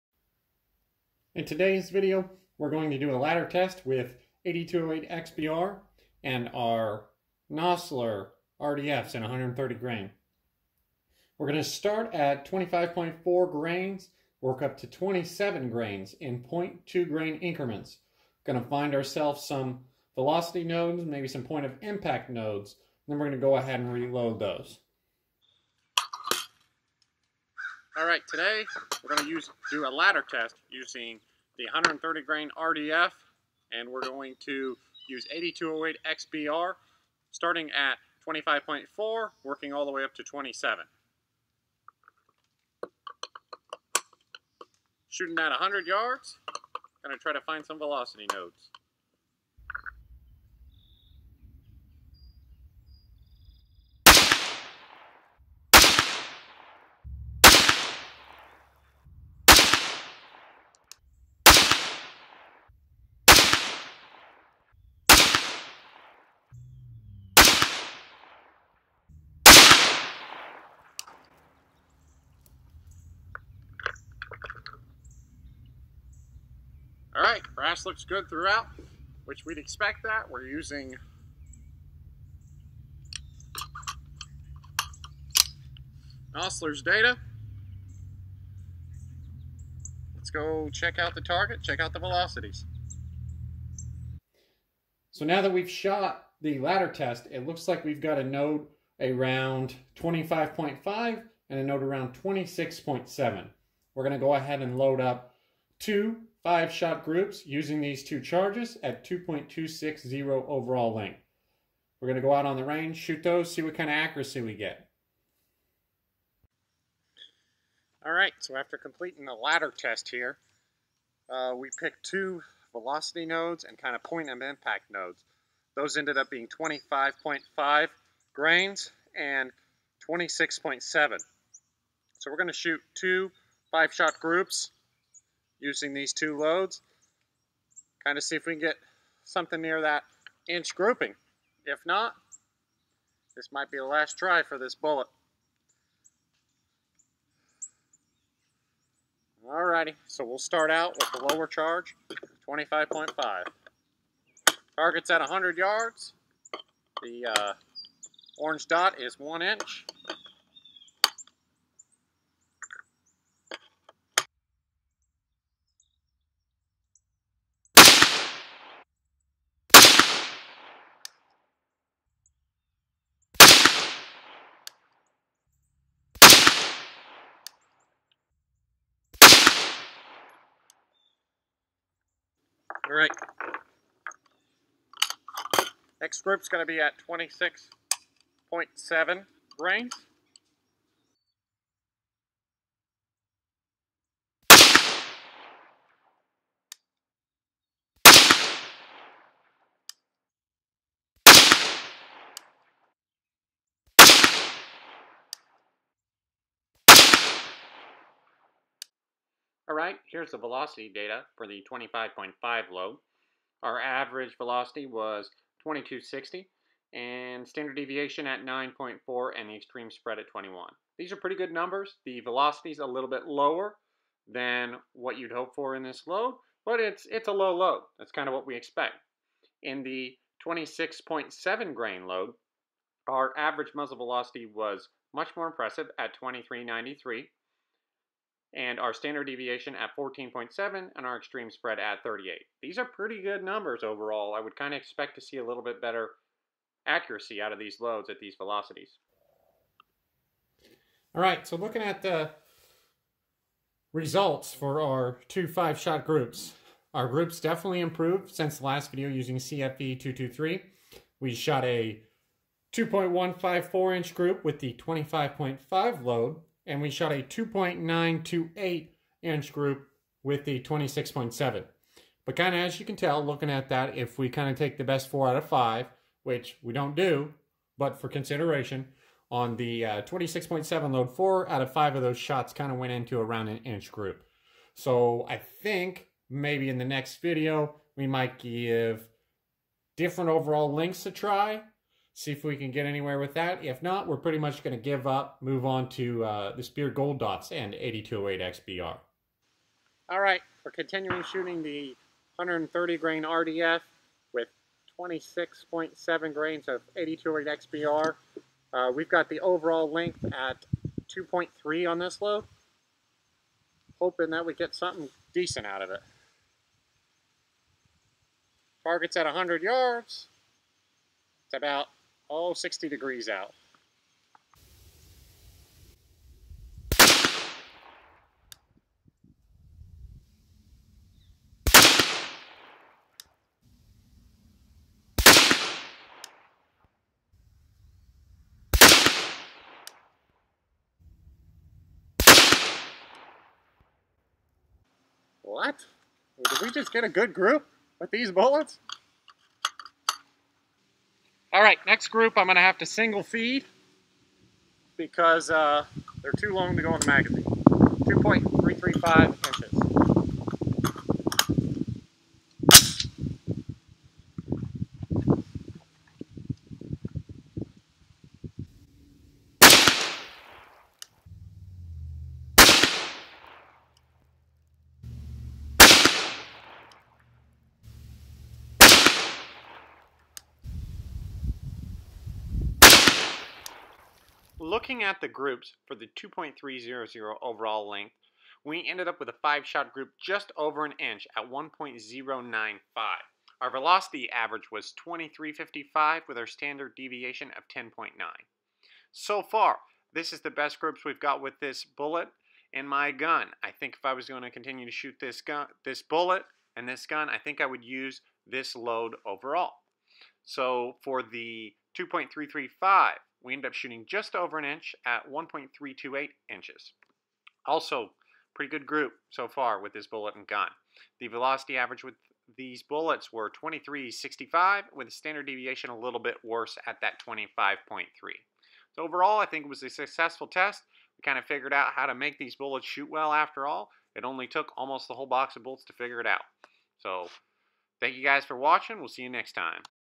How to Get Accuracy with 130gr Nosler RDF in 6.5 Grendel
Load work up including ladder testing, and trying different OAL until finally finding a group that shoots near 1 MOA at 100yds in my rifle. Nosler 130 rdf have a history of not grouping well in Grendel likely due to their length and significant jump at magazine length. This video shows the process to find a decent load using the 130gr RDF and 8208 xbr in my 20 inch 6.5 grendel AR15. Best groups featured at the end of the video.